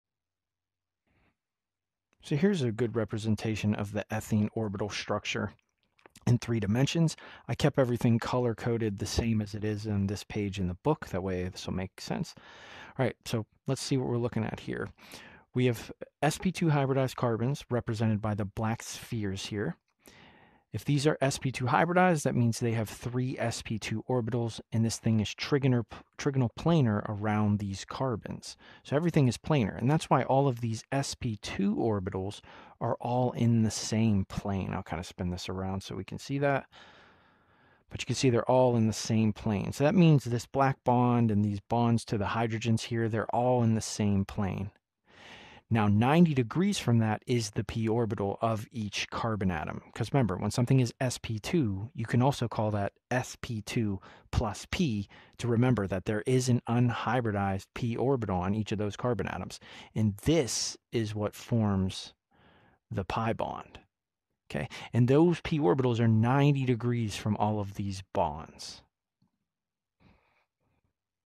Orbital Model of Ethene in 3-Dimensions from page 58 of Will’s Ochem 1 Notes
Orbital model of ethene from page 58 of Will’s Ochem 1 Notes